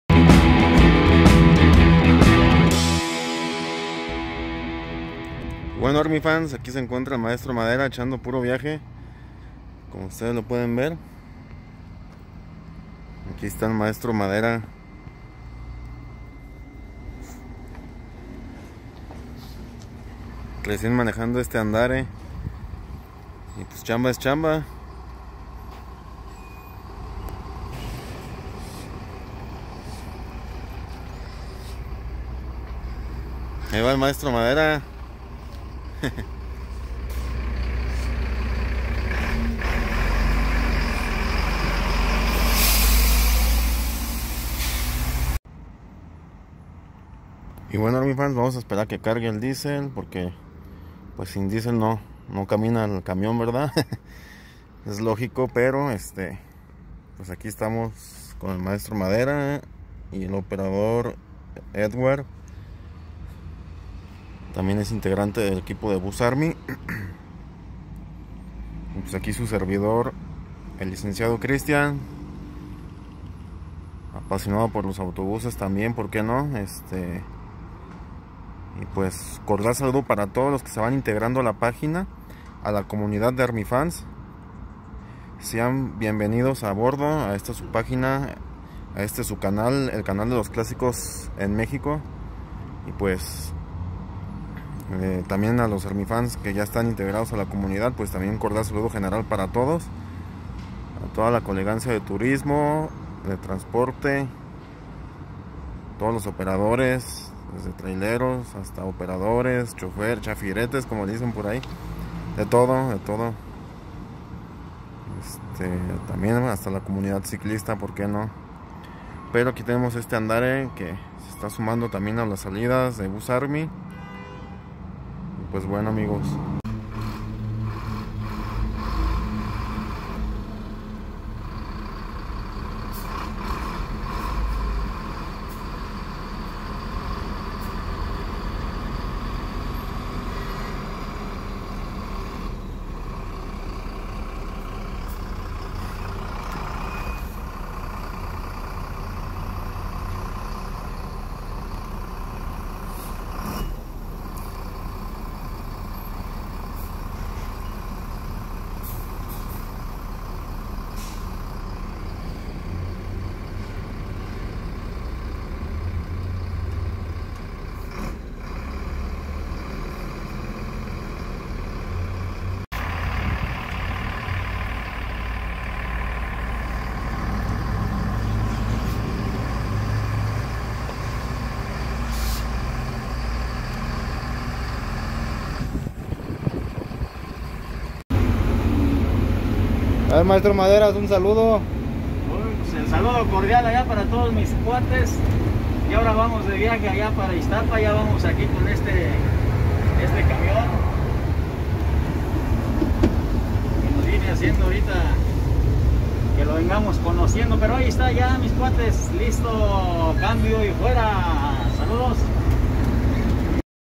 Andare Mercedes-Benz y Mtro. Maderas al manejo
Qué tal ArmyFans, alguna vez han viajado en estos autobuses Andare?, comenten en que línea de autobuses lo vieron...